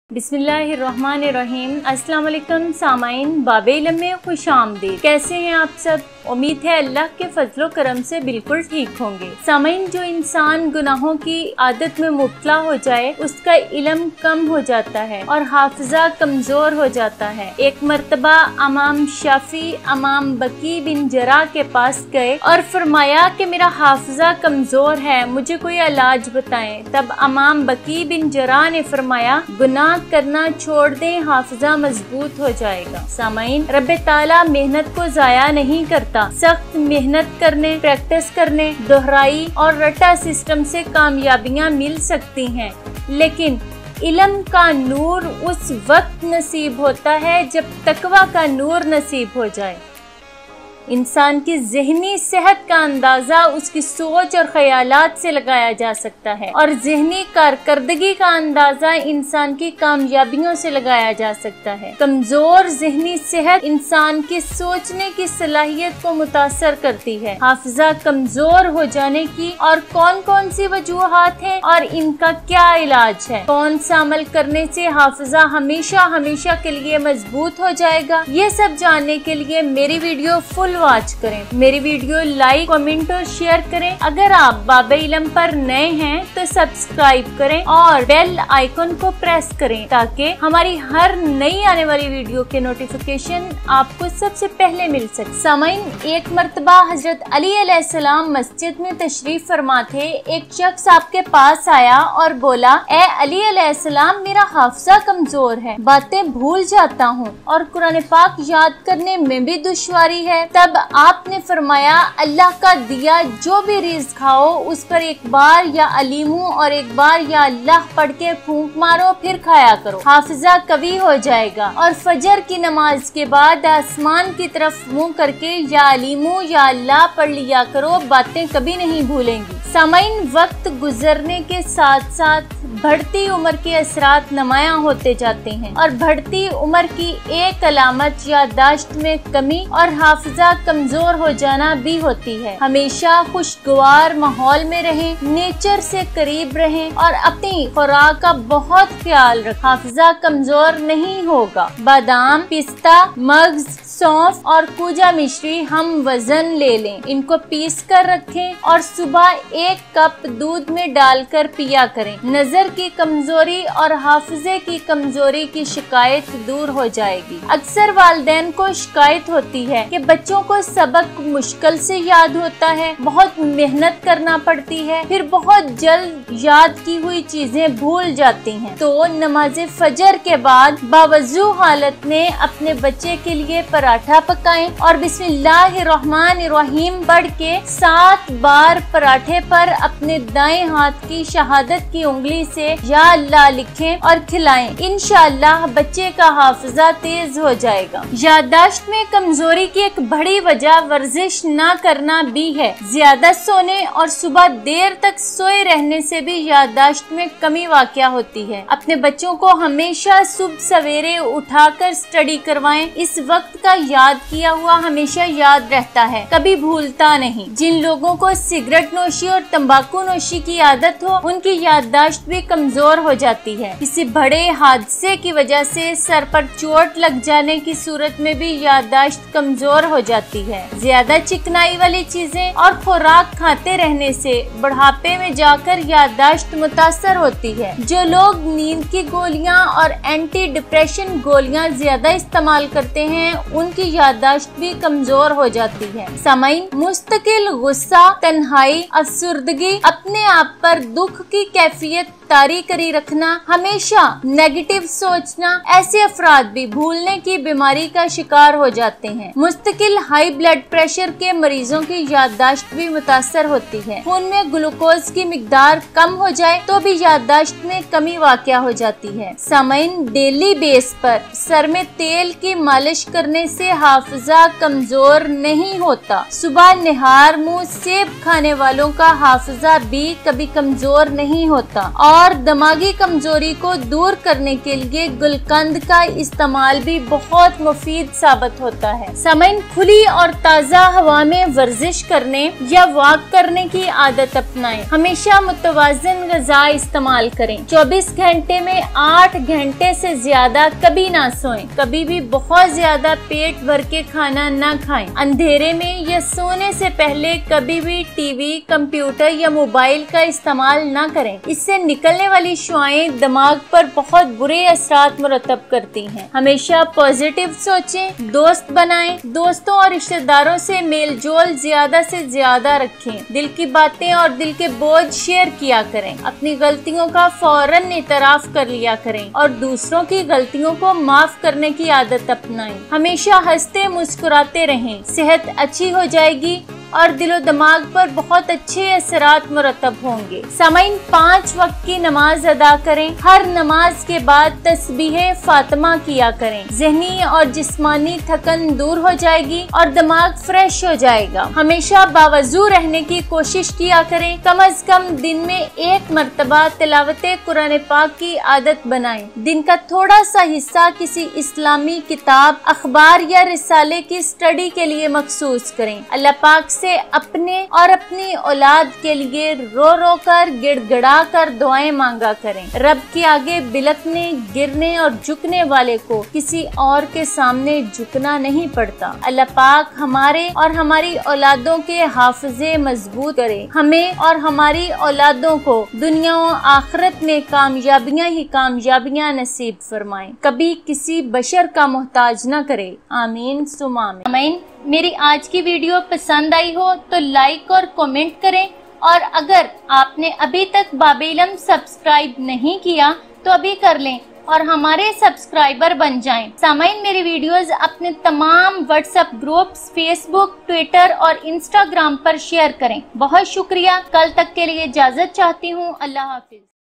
How we improve our memory and Brain power || dimag tez karne ka tarika\ Wazifa
Powerful tips and techniques to improve your IQ and memory power. This video on brain power in hindi will help you to increase your focus and concentration along with your mind power.